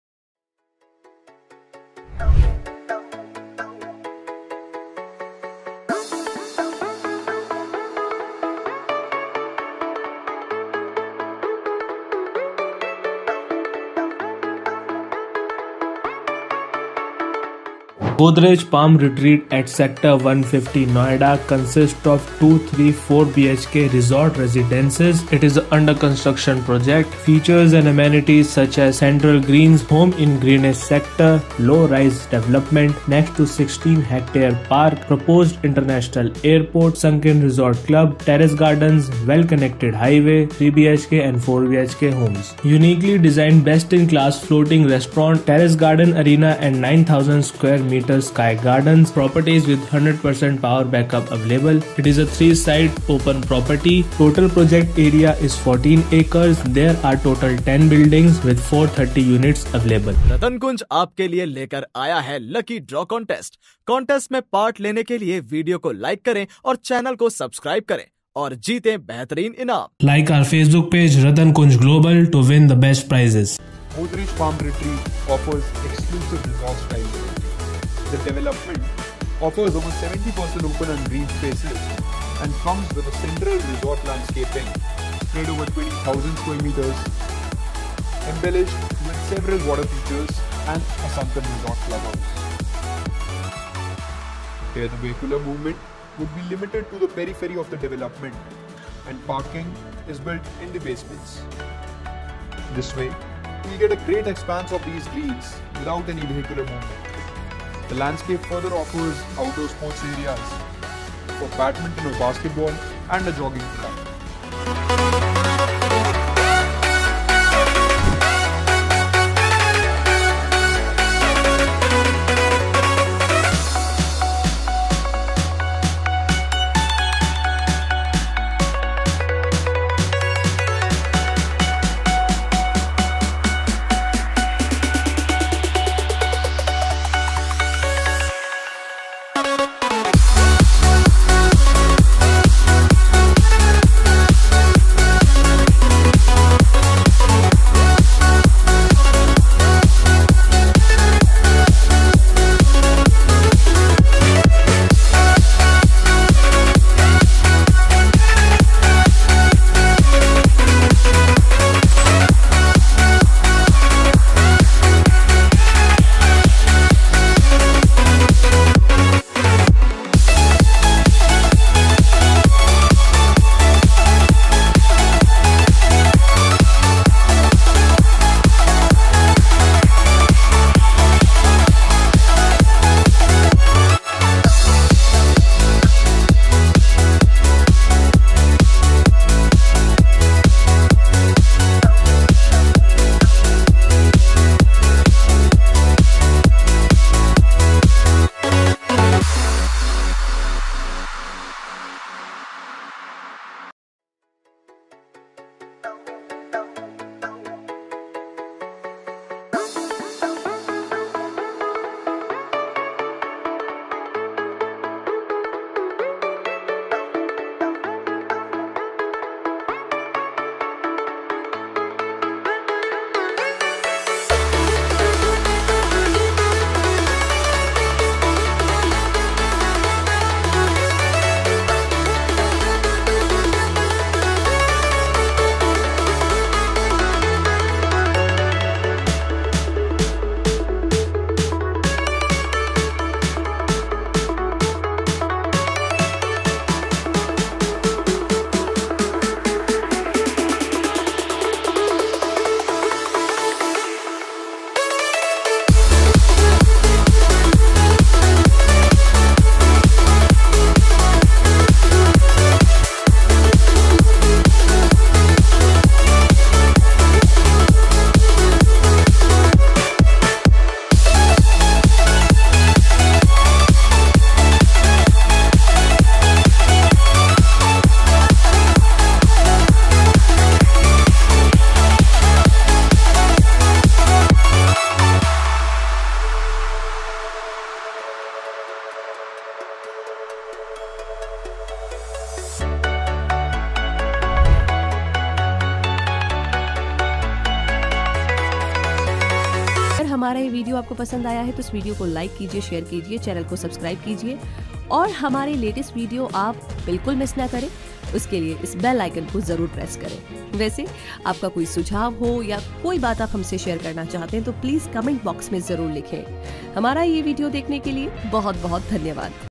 Godrej Palm Retreat |☎️#8130084691 For Site Visit | Sector-150 Noida | 2,3,4 BHK Resort Residences
Features & Amenities-

✨ Central Greens
✨ Home in the Greenest Sector
✨ Low Rise Development
✨ Next to a 16 Hectare Park
✨ Proposed International Airport
✨ 3 BHK & 4 BHK Homes

Project Highlights:-

✔  9 hole golf course and international cricket academy nearby
✔  Uniquely designed best in class floating restaurant and multi-level pool
✔  Well Connected Highway

⭐ Configurations - 2, 3, 4 Resort Residences
⭐ Project Area - 11.43 Acre
⭐ Project Status - Under Construction